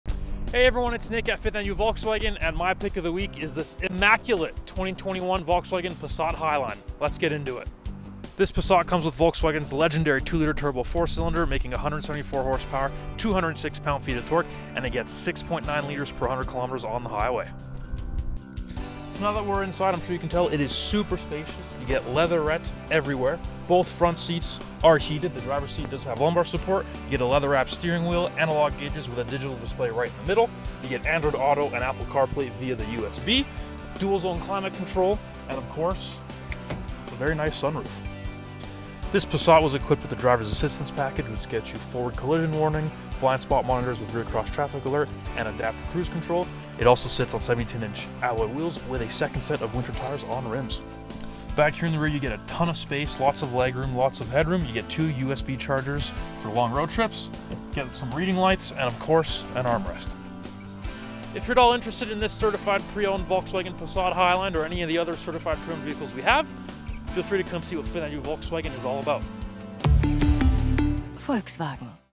2020 Volkswagen Passat Highline | Car of the Week | Fifth Avenue Volkswagen
Rev up your engine and get ready for an exhilarating ride with this week's car of the week, our immaculate 2020 Passat Highline! This turbocharged beauty boasts a powerful 2 litre 4 cylinder engine that delivers a whopping 174 horsepower and 206 pound feet of torque, giving you that much-needed adrenaline rush on the road.

But that's not all! Our Passat Highline is also fuel-efficient, giving you a smooth ride that's easy on the pocket. With 6.9 litres per 100 kms on the highway, you can go on longer drives without worrying about frequent refueling stops.

Plus, with 17 inch alloy wheels, you'll be riding in style and turning heads wherever you go. And for those colder months, we've got you covered with a second set of winter tires on rims, ensuring you'll be able to drive smoothly and safely no matter the weather.

Don't miss out on this one-of-a-kind driving experience! Visit us today to take our immaculate 2020 Passat Highline for a spin.

1120 Meridian Rd NE,
Calgary, Alberta, T2A 2N9